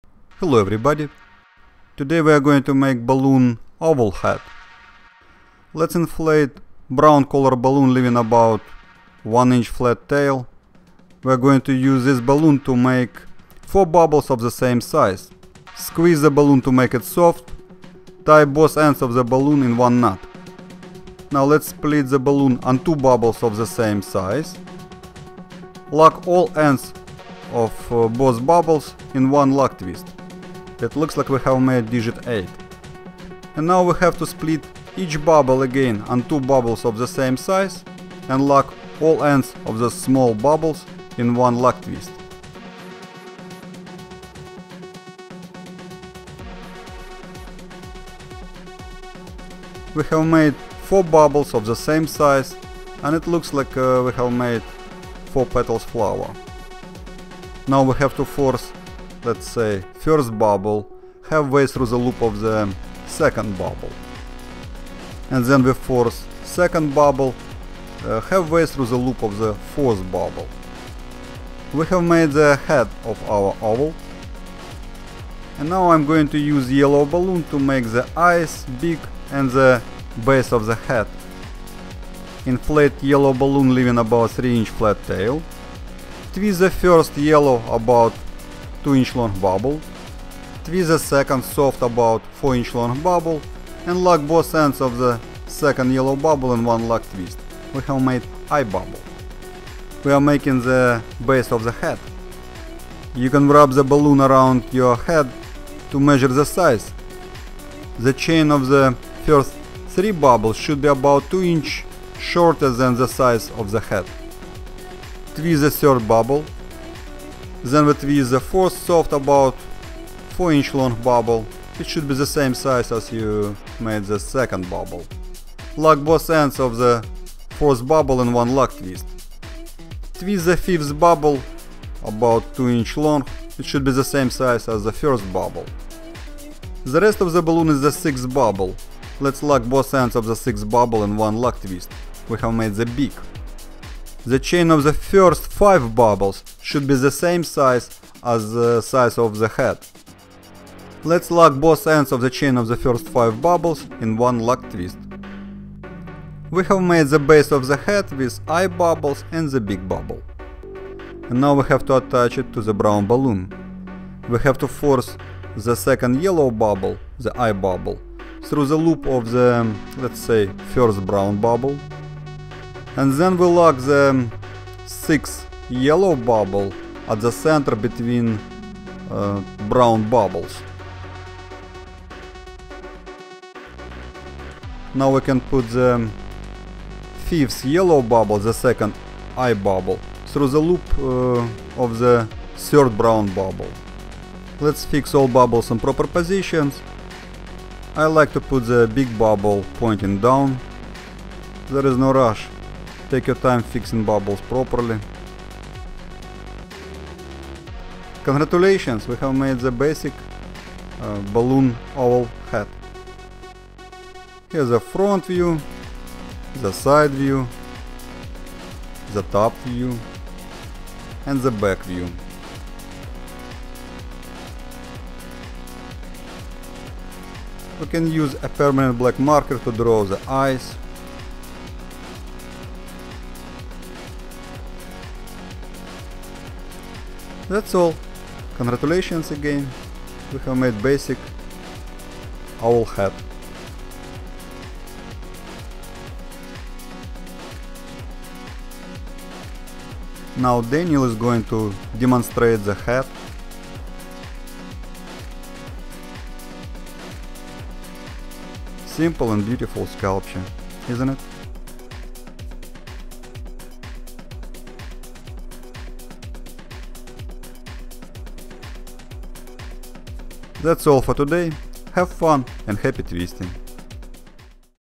Balloon owl hat
In this video instruction you will learn how to make balloon owl hat using balloon twisting technique.

Step by step instructions with photos and text on blog post